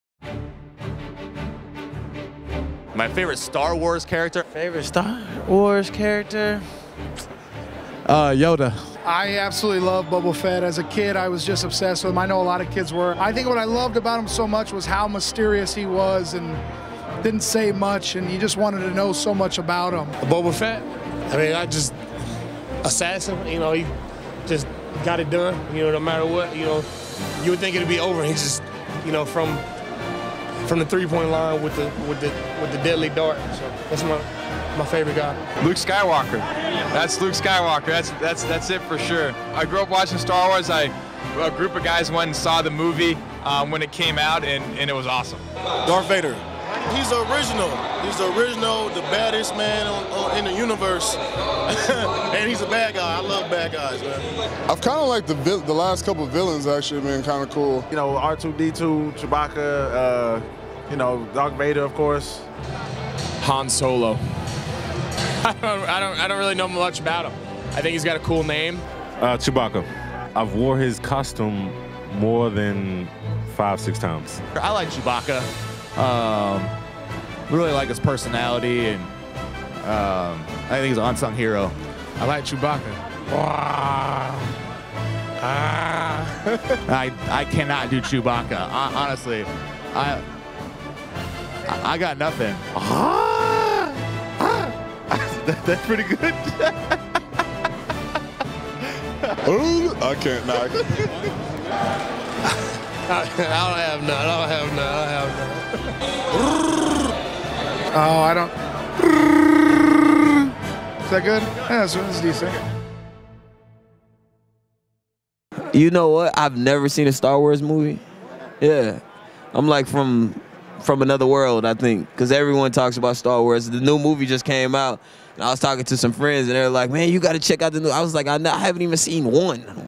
Super Bowl Stars Select Favorite Star Wars Characters
Check out some of the biggest stars in the Super Bowl sharing their favorite Star Wars character and then doing their best Chewbacca impression.